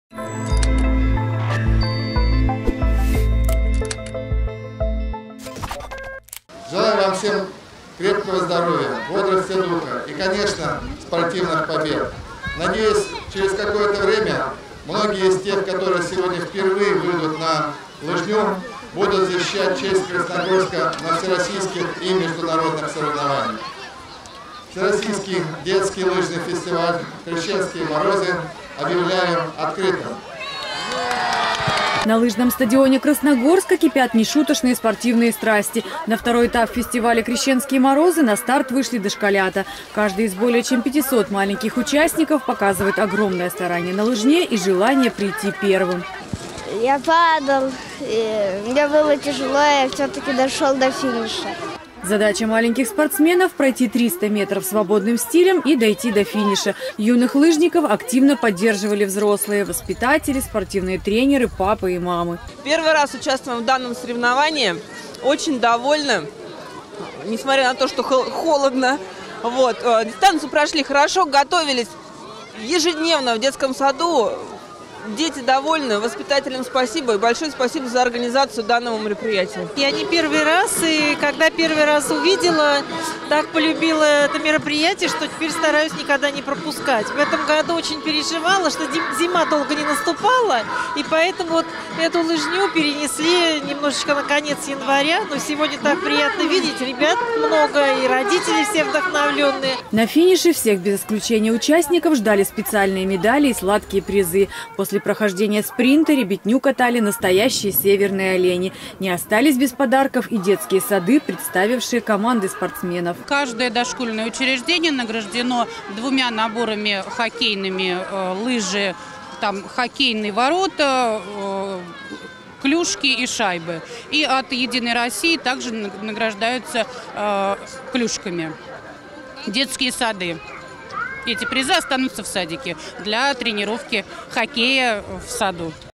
КРТВ. Второй этап фестиваля «Крещенские морозы»
На лыжном стадионе Красногорска кипят нешуточные спортивные страсти – на второй этап фестиваля «Крещенские морозы» на старт вышли дошколята. Каждый из более чем 500 маленьких участников показывает огромное старание на лыжне и желание прийти первым.